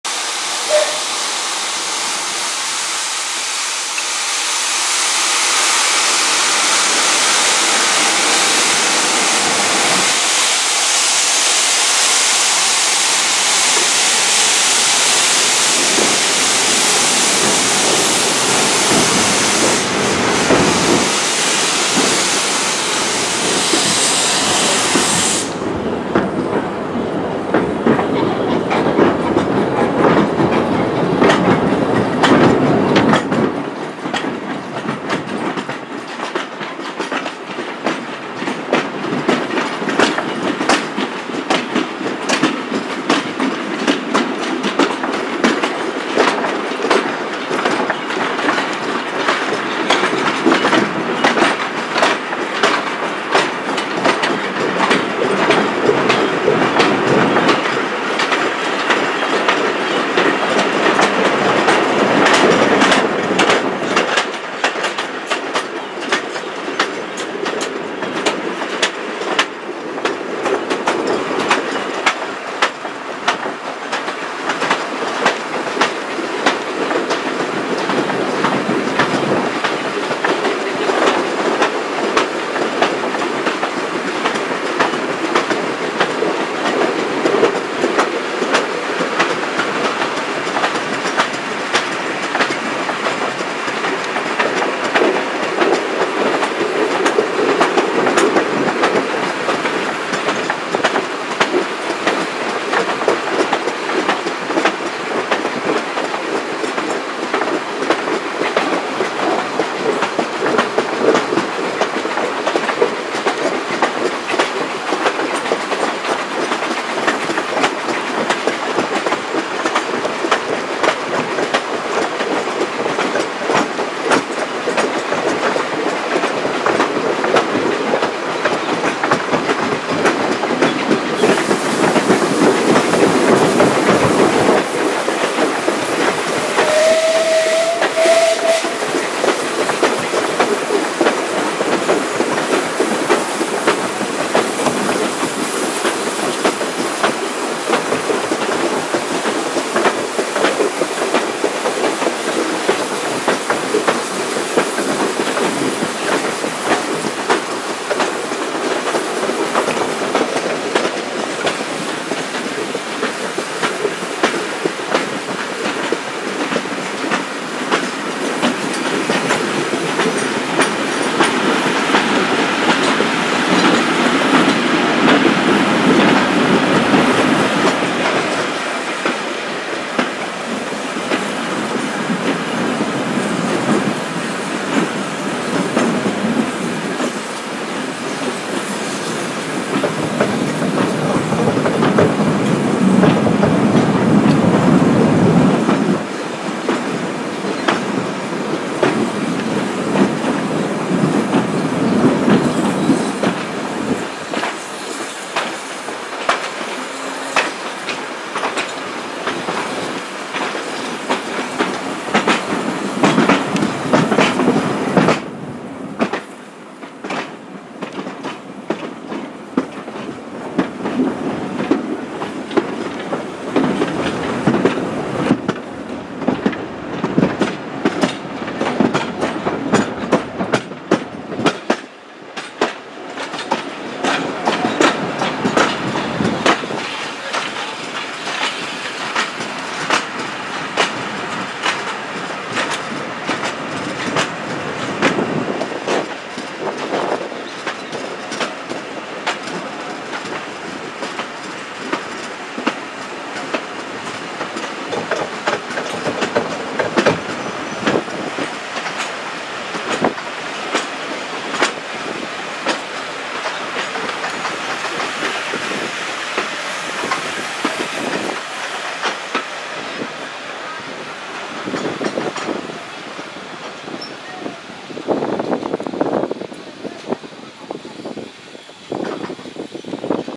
From Butterley to Swanwick aboard vintage coaching stock and Y7 985 @ Midland railway centre 4/5/15
first ride of the day from Butterley to Swanwick junction onboard the vintage coaching stock set and behind LNER Y7 985